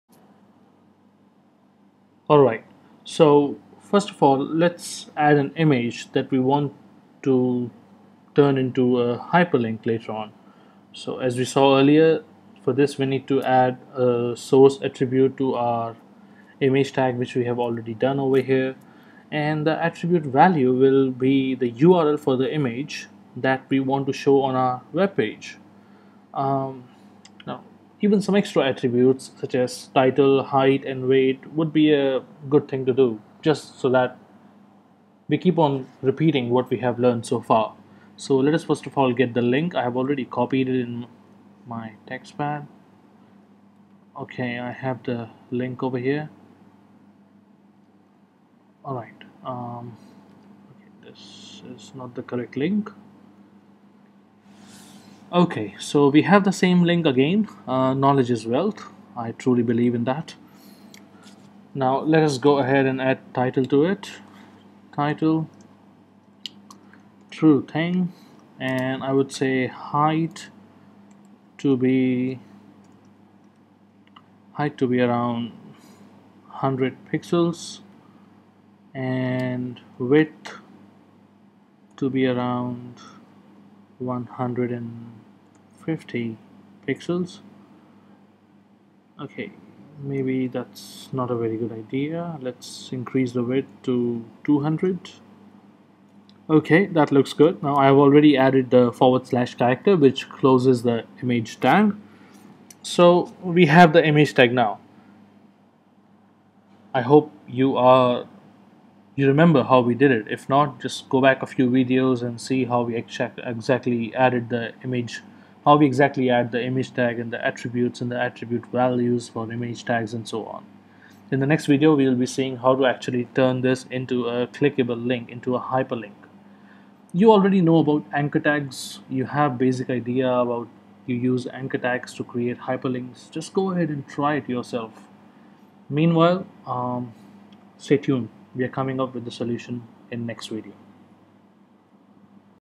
HTML 19 - Add an image to turn into hyperlink
In process of creating a clickable image. First adding an image to our webpage and then in next video we will see how to add anchor tag to turn that image into a hyper link.